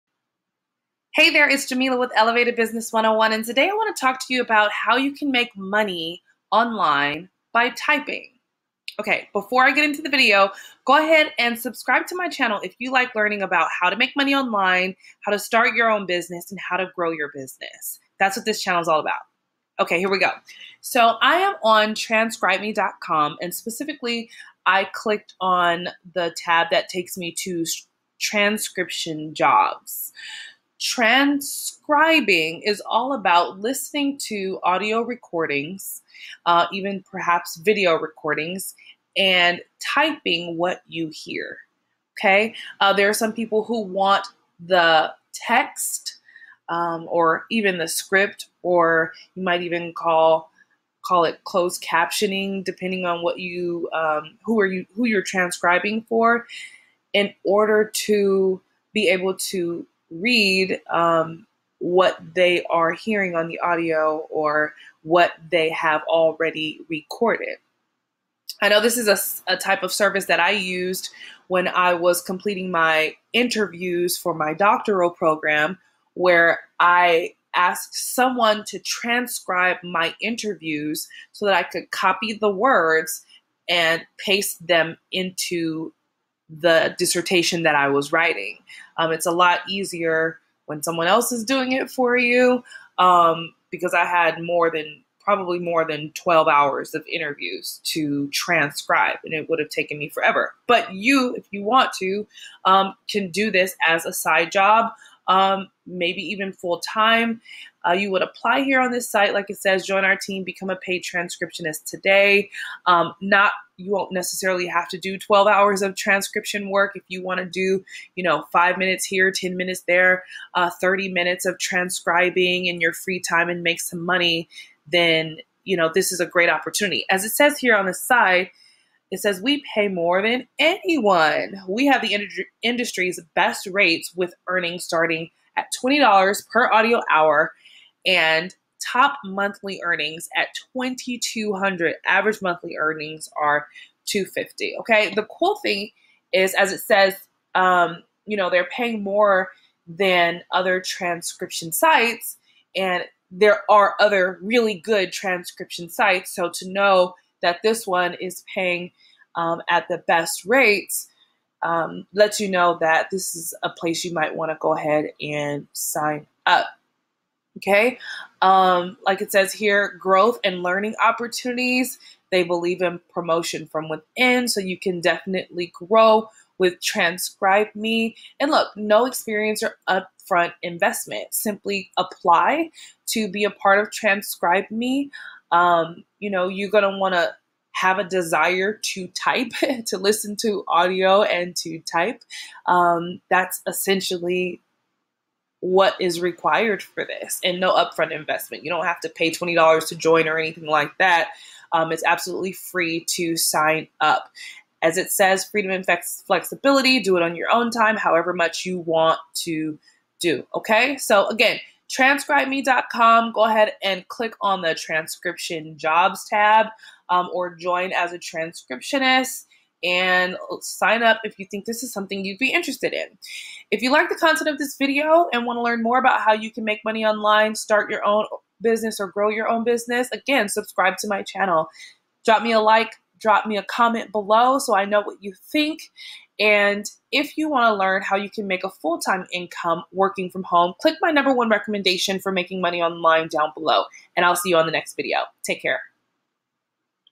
How to Make Money Online by Typing in 2018
In this video, I introduce you to the opportunity to do transcription work with TranscribeMe! No experience necessary, and TranscribeMe! boasts of being the industry's highest paying transcription site. Watch to learn more.